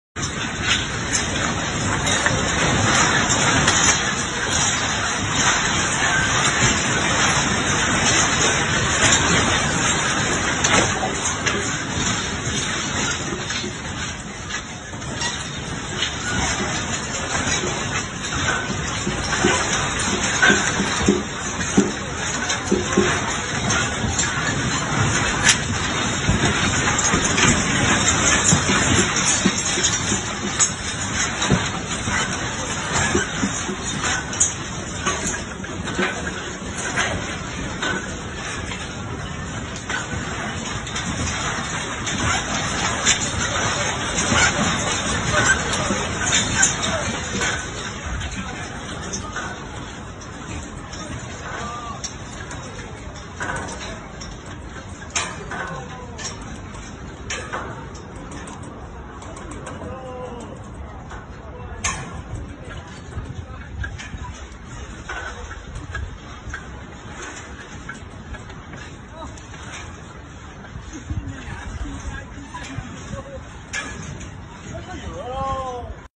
ekuador di hantam gempa M6,8 pada hari sabtu 18/3/2023
ekuador dilanda  gempa M6,8 pada hari sabtu 18/3/2023